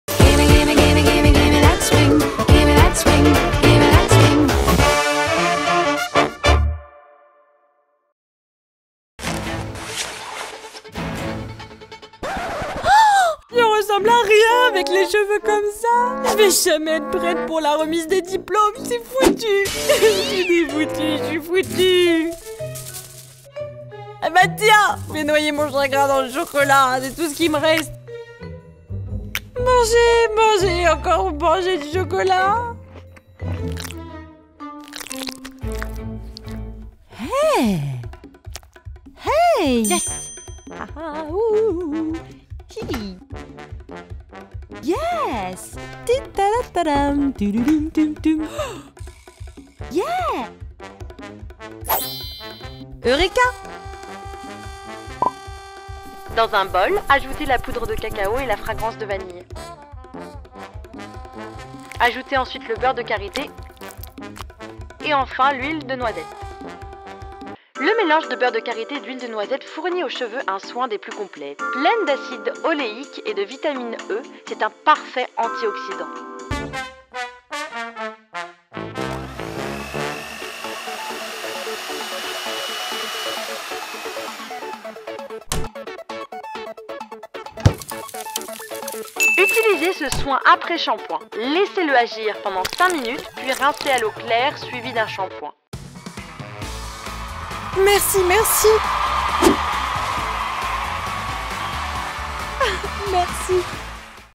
Masque capillaire au chocolat - Cosmecook
Ça hydrate le cuir chevelu et c'est au chocolat ! Quoi de mieux ?